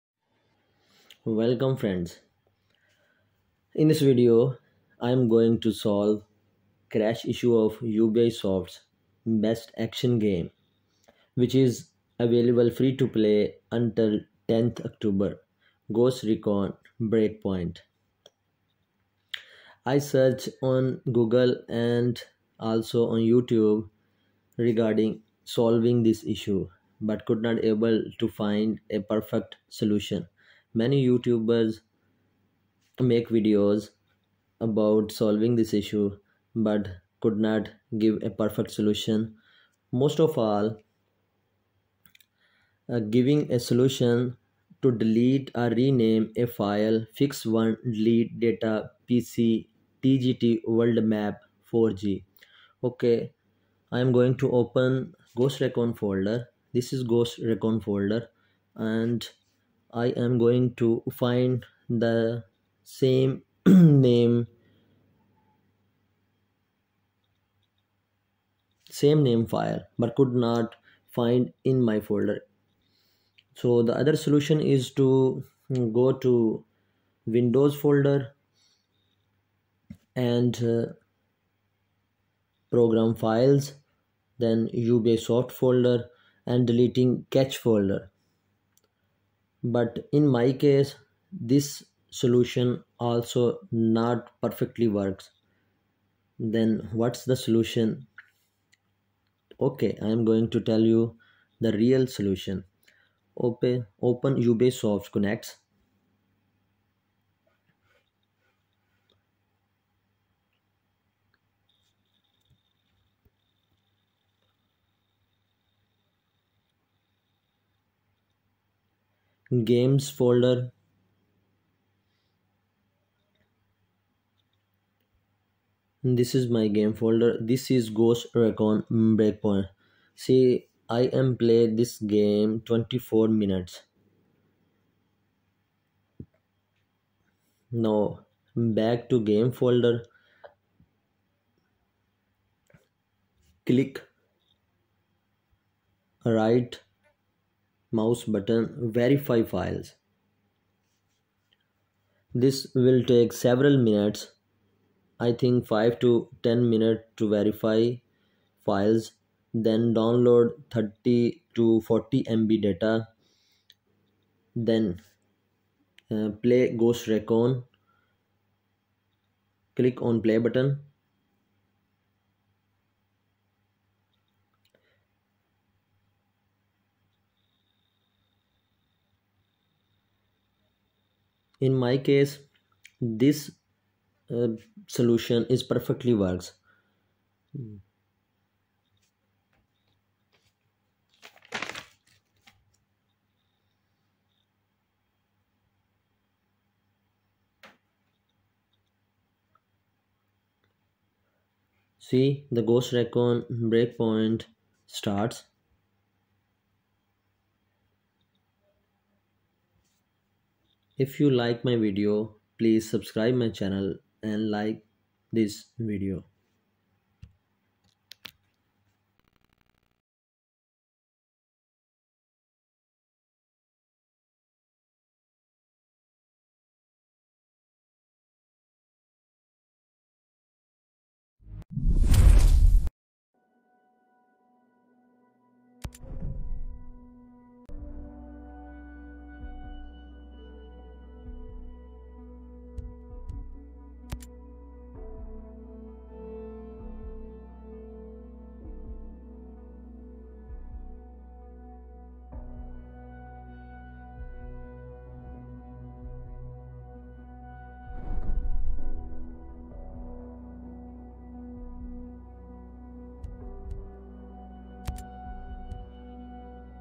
Ghost recon crash solution 2022
This video is about solving crash of ghost recon break point.

ghost recon crash
ghost recon glitch
ghost recon breakpoint crash
how to solve crash
how to solve ghost recon breakpoint crash
fix issue